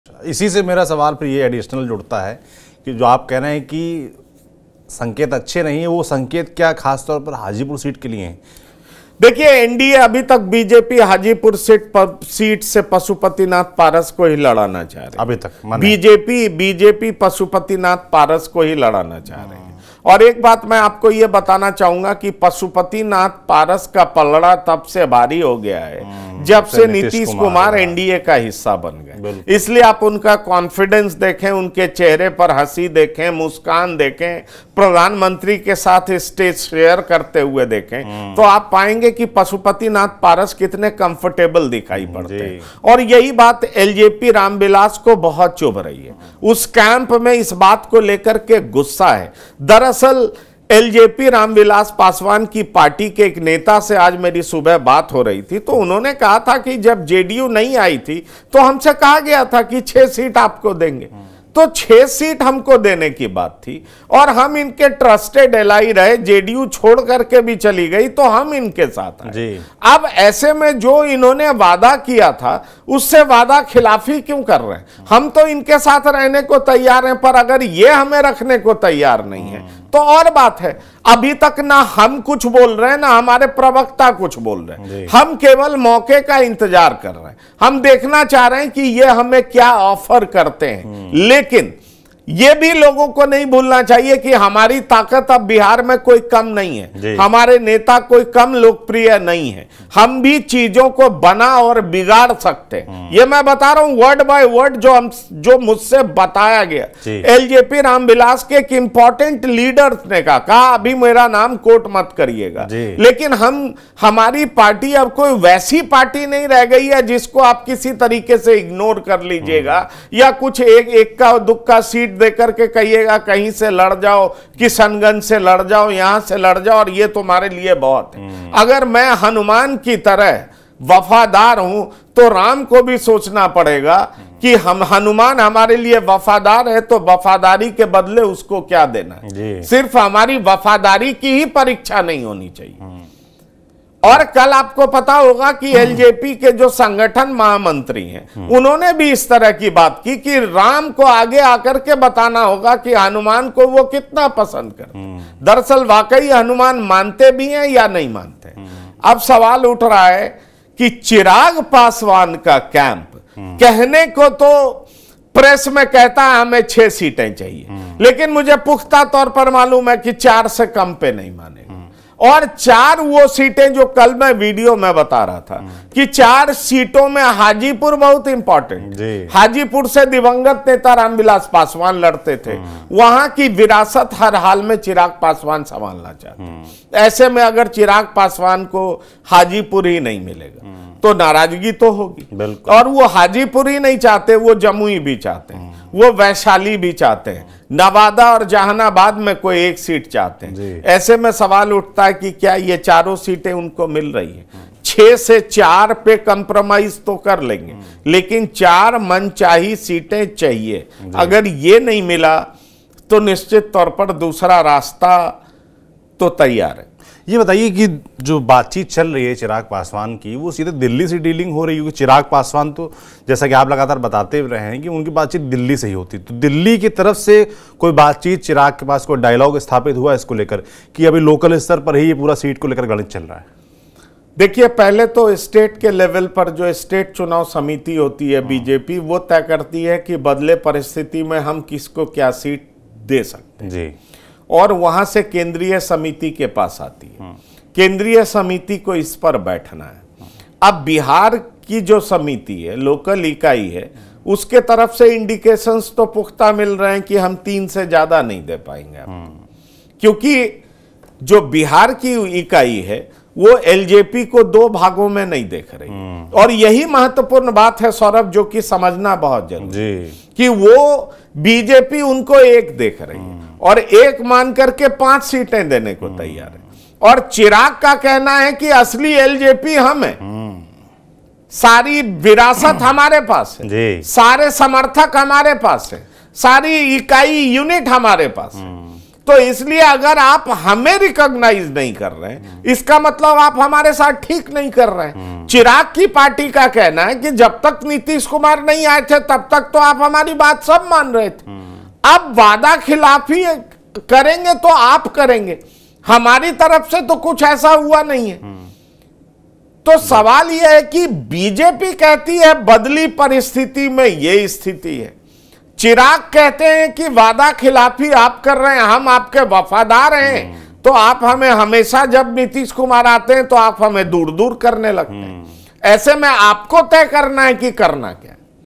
Chirag Paswan से बाजी छीन ले गए चाचा Pashupati Paras? NDA किससे पक्ष में खड़ी है?
हमसे जुड़िए.